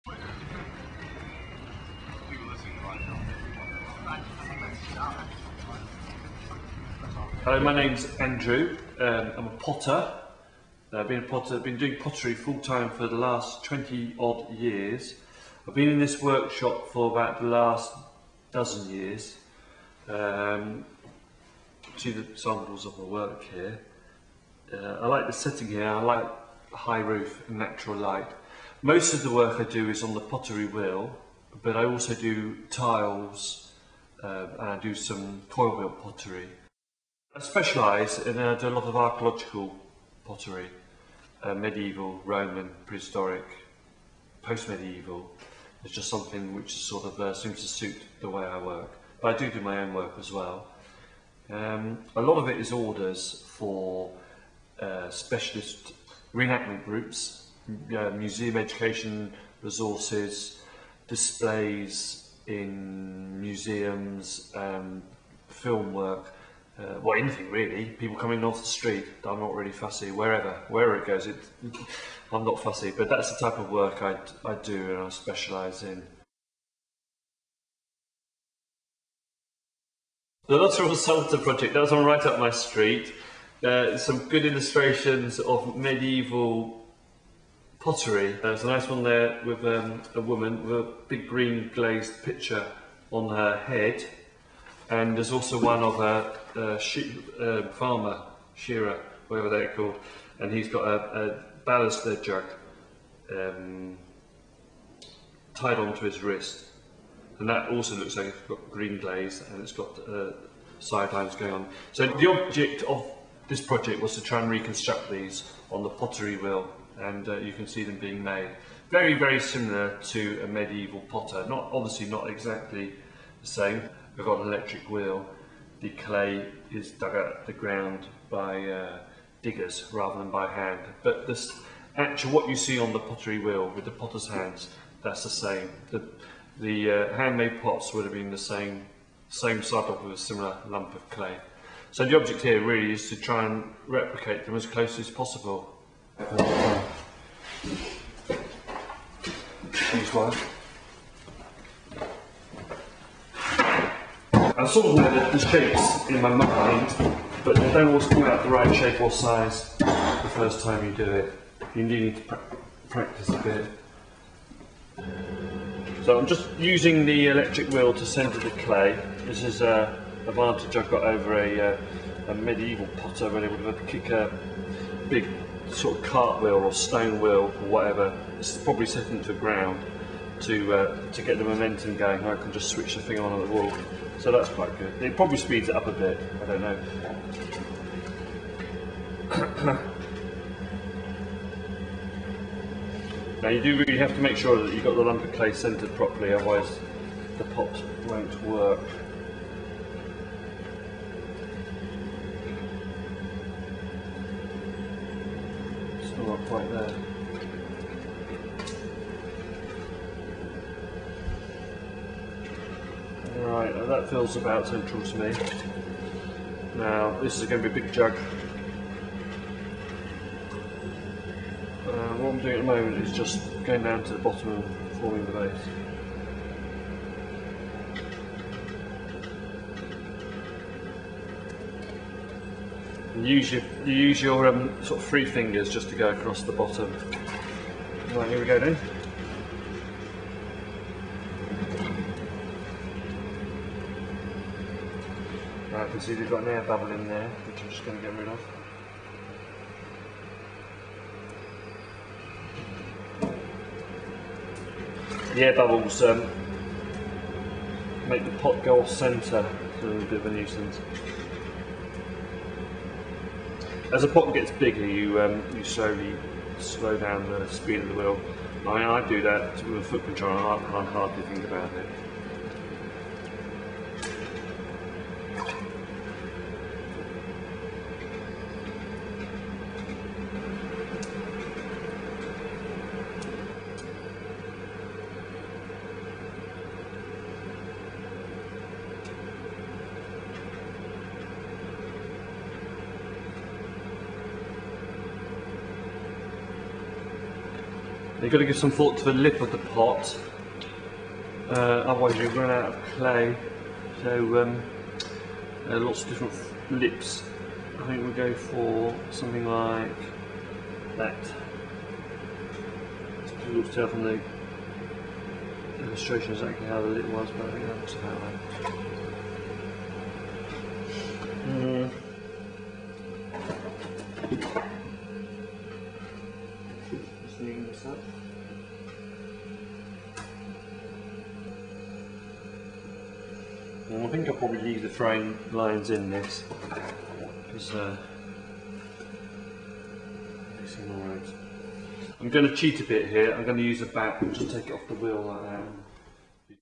Luttrell Psalter: Medieval pottery Part One
Specialist potter Andrew MacDonald recreates some medieval pottery as seen in the Luttrell Psalter. Part one: throwing.